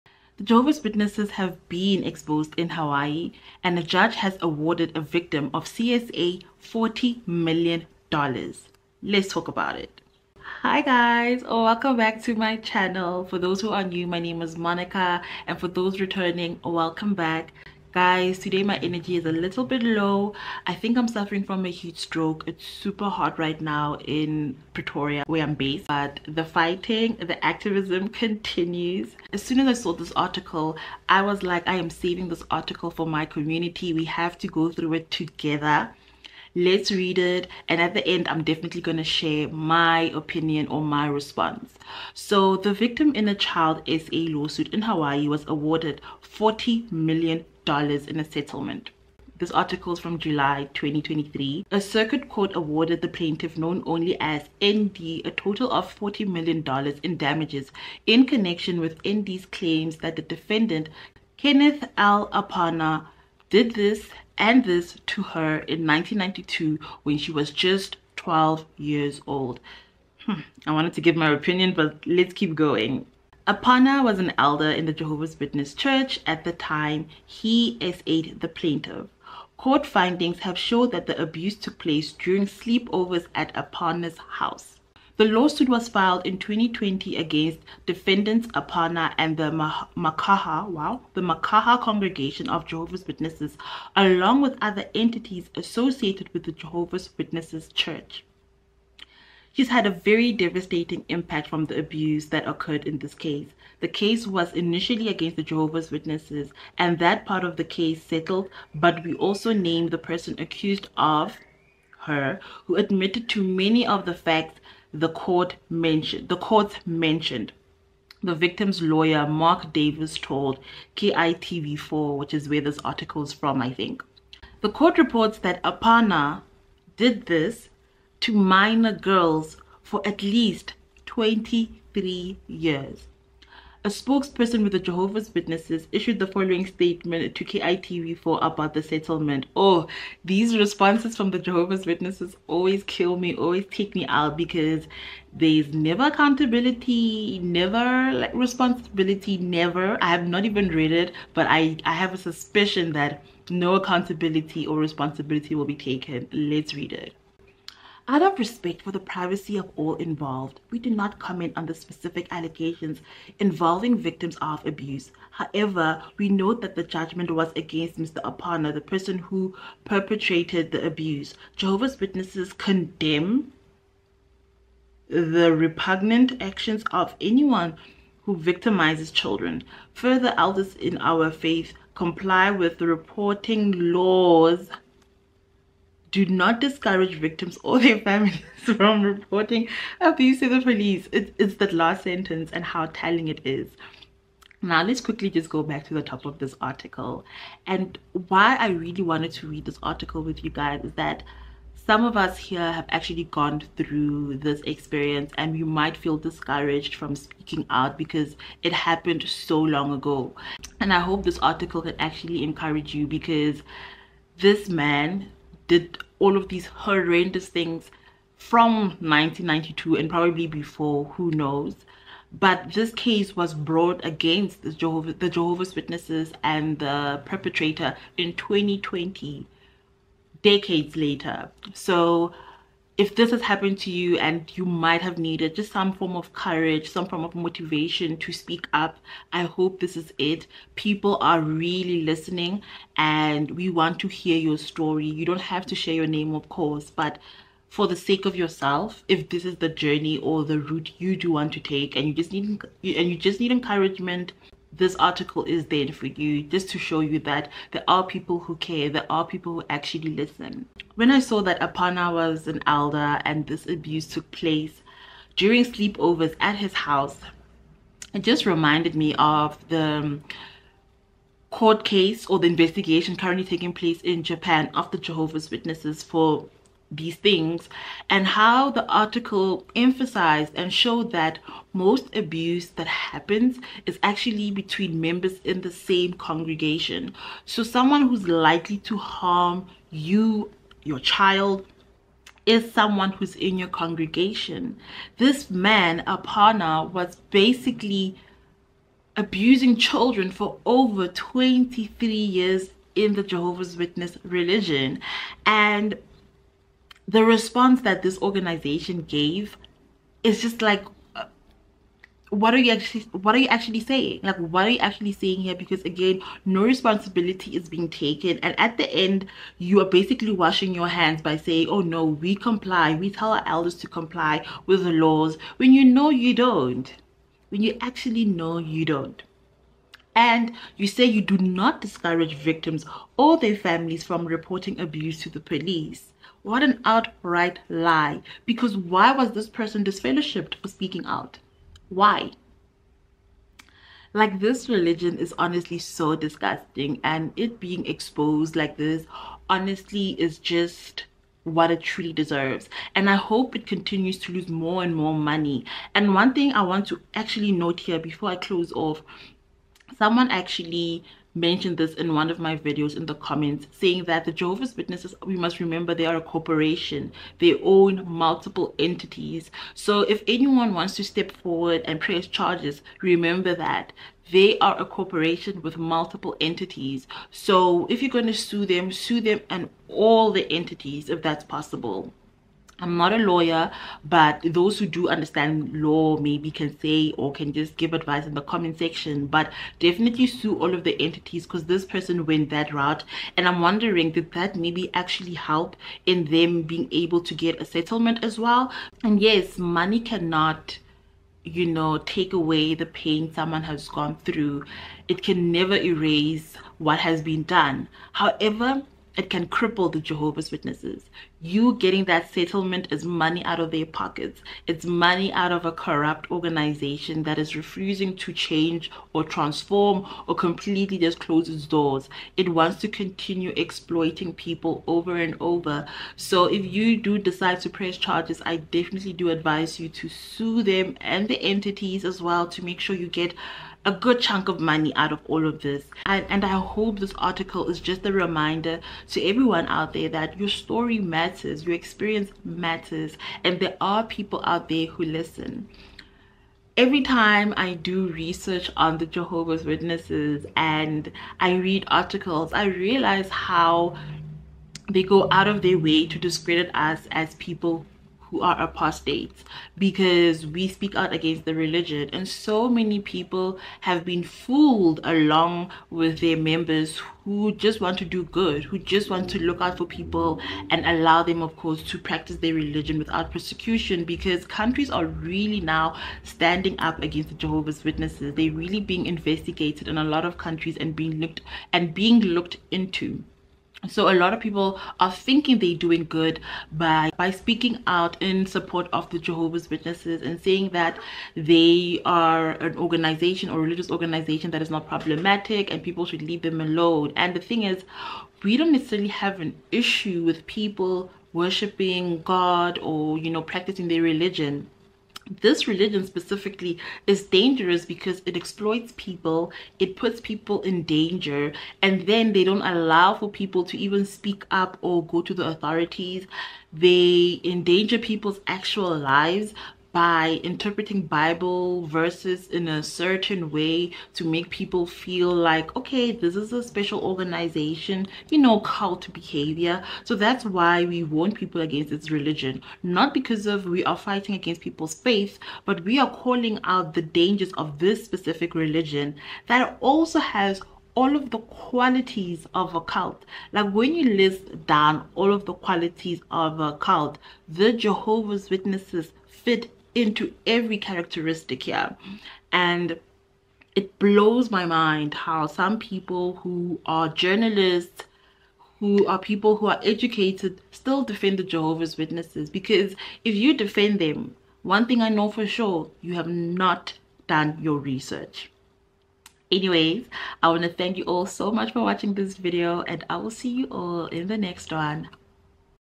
Exposing Jehovah’s Witnesses: Scandal in Hawaii
The Jehovah’s Witnesses were exposed in Hawaii after a court ordered a $40 million settlement to a child abuse victim.